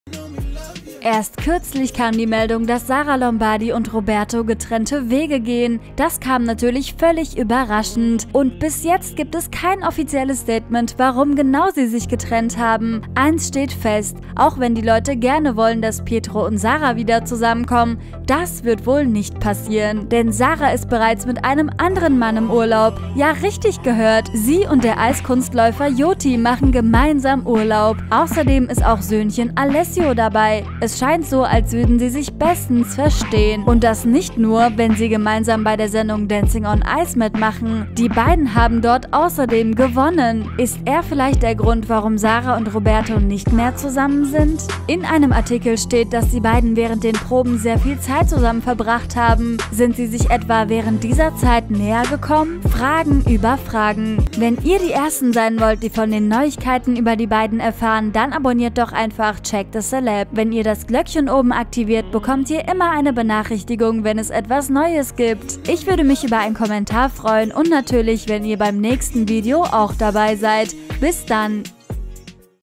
JOTI & SARAH LOMBARDI das neue Liebespaar?! ❤ Trennung und direkt den neuen an der Seite!
* Hier zum 99 Cent Musik Angebot
4 Monate fast umsonst

Infos zum Video:
SARAH LOMBARDI trennt sich von Ex-Freund ROBERTO und nur wenige Tage später ist sie mit ihrem Tanz-Partner JOTI in Ägypten unterwegs und macht Urlaub. Romantische Fotos sind dabei entstanden und alle fragen sich ist er der neue an ihrer Seite?!